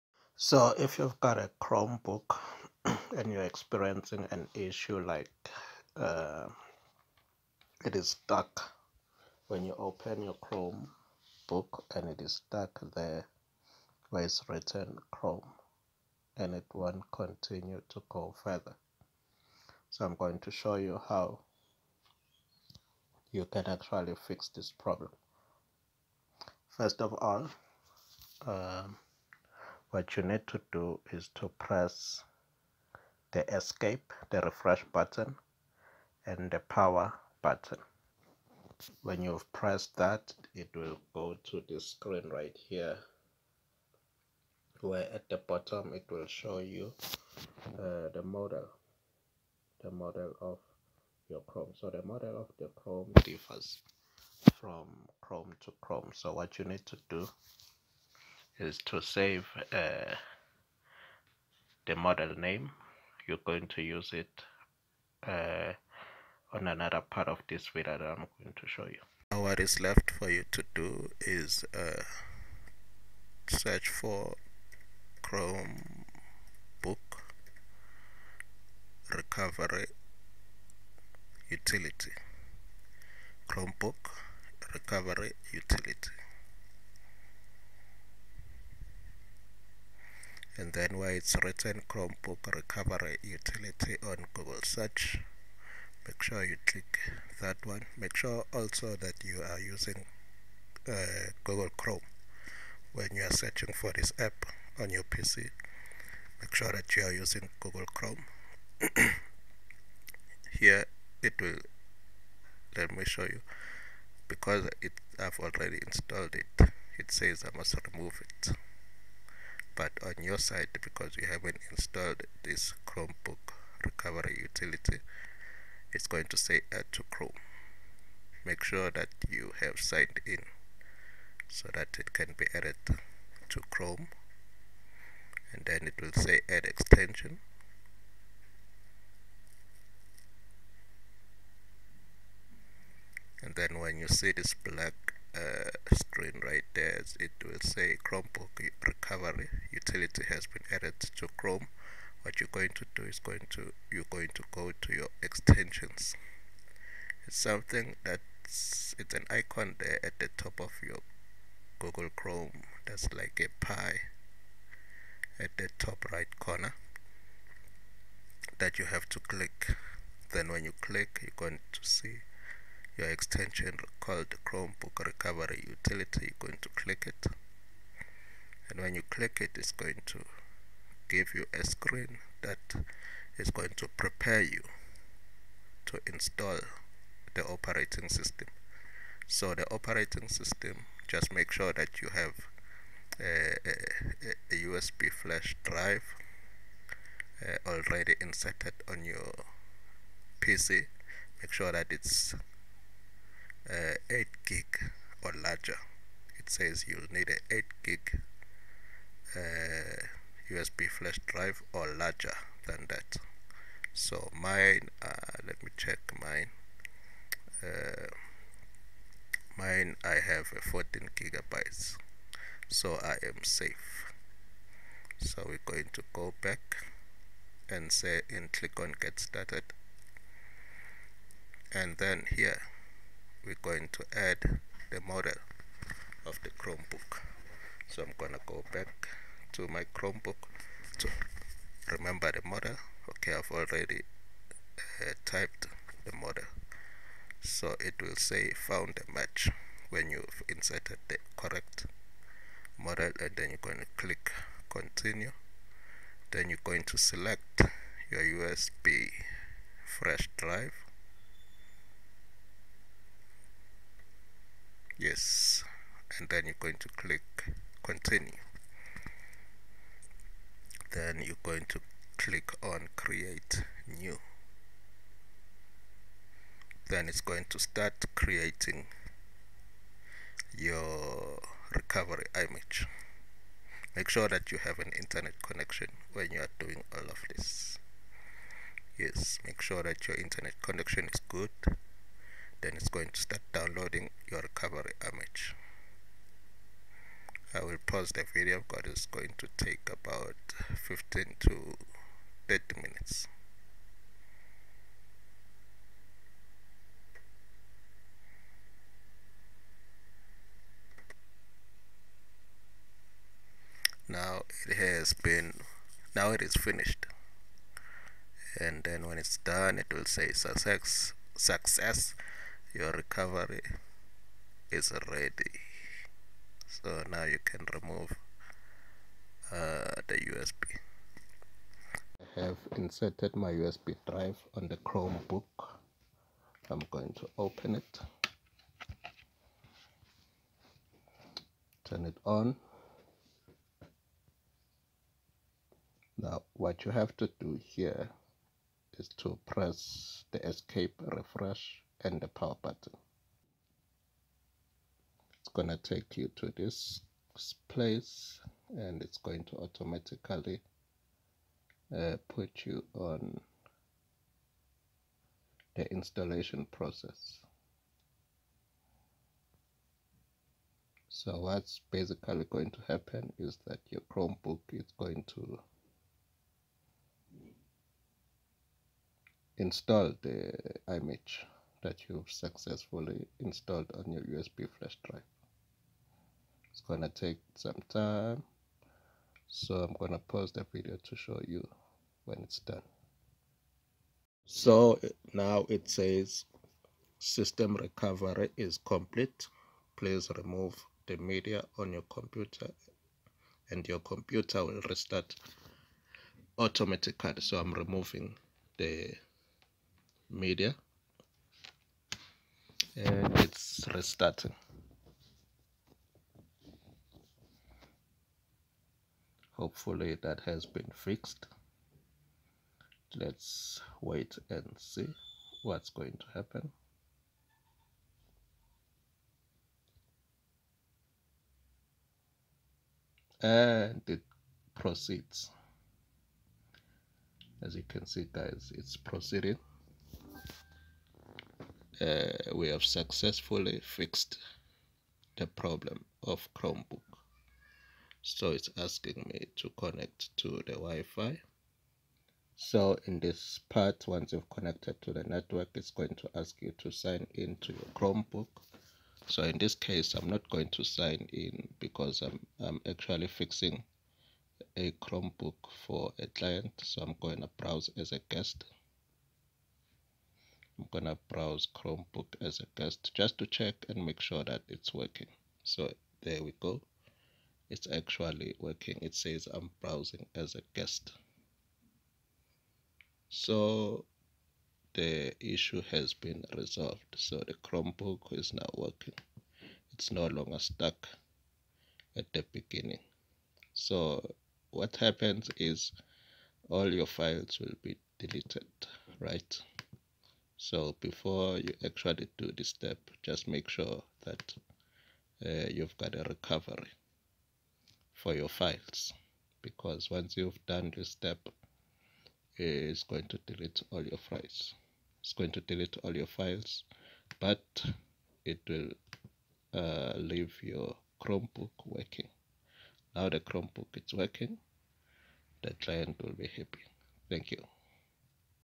how to fix chromebook stuck at chrome start window
In this video, I am showing you an easy way to fix Chromebook that is refusing to start and stuck a start window. Make sure you have another pc and a USB flash drive.